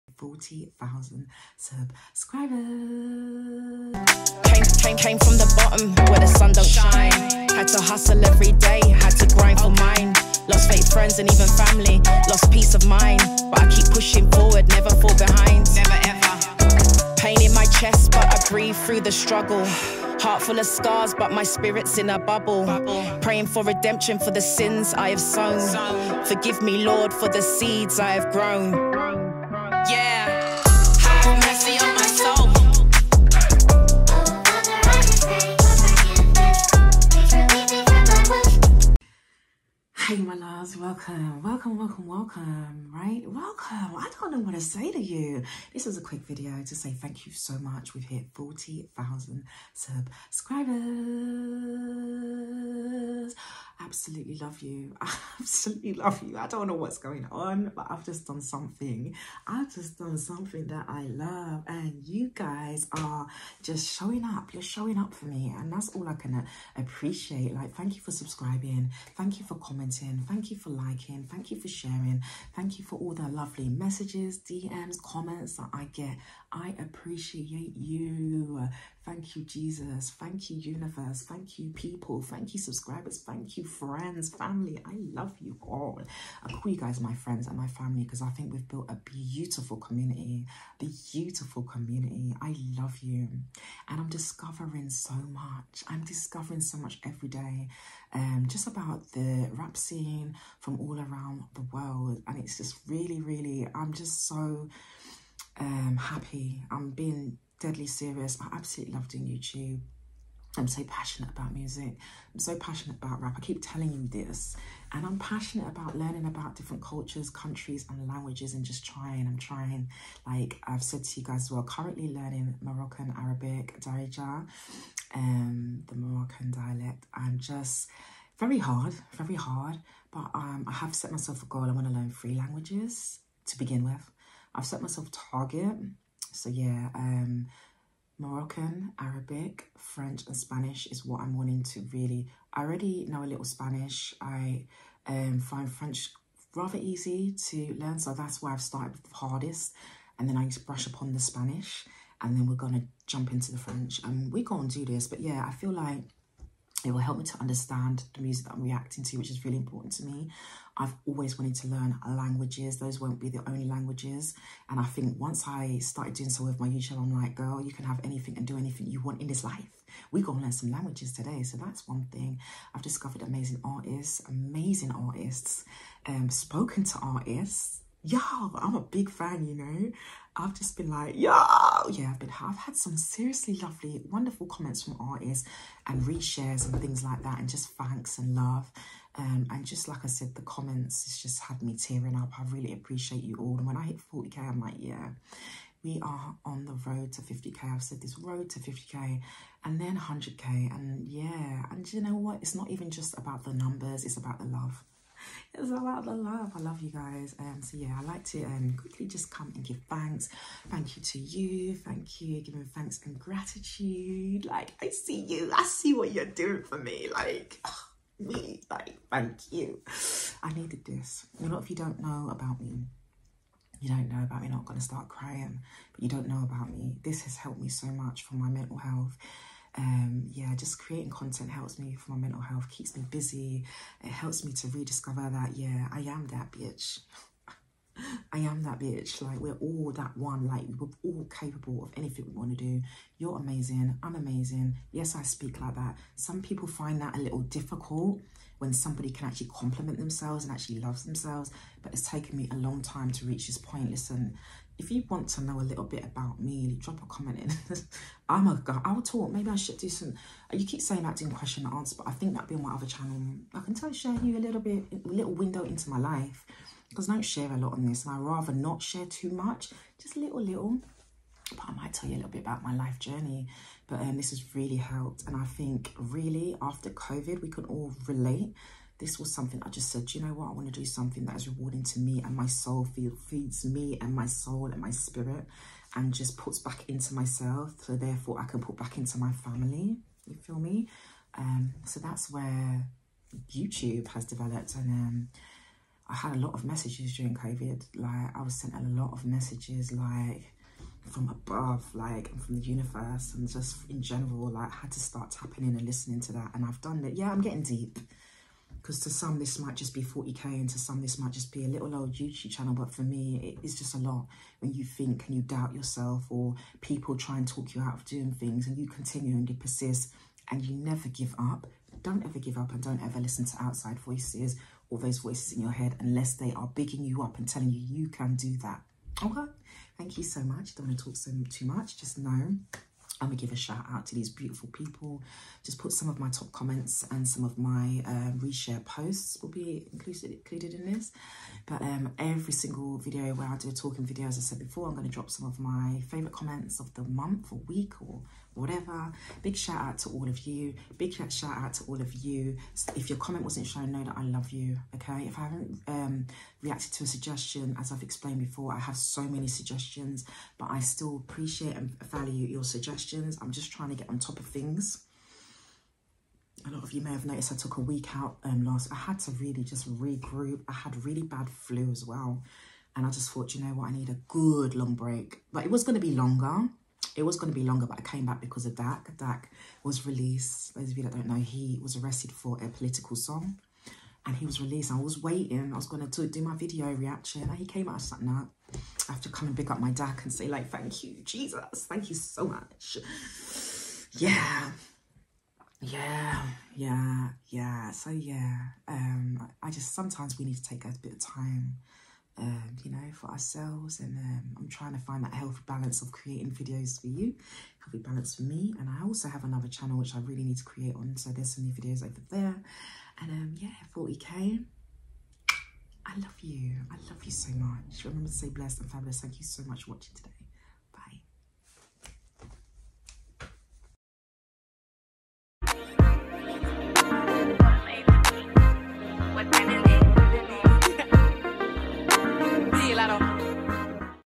40k Subscribers! I Luv You… Shout out to my subscribers 🖤
Thank you all so much for helping me to reach 40k subscribers

I honestly appreciate all the luv and support and I am so thankful

Big shout out to you 🖤🖤🖤🖤